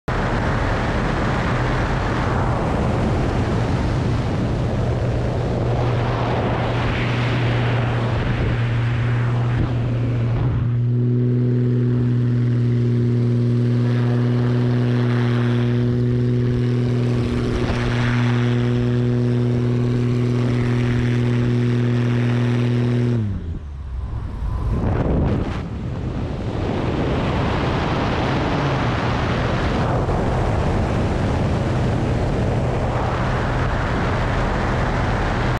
A little fun in the Extra 300
My attempt at a spiraling tower. Not enough forward stick and opposite aileron to really get the snap out of it. Better luck next time!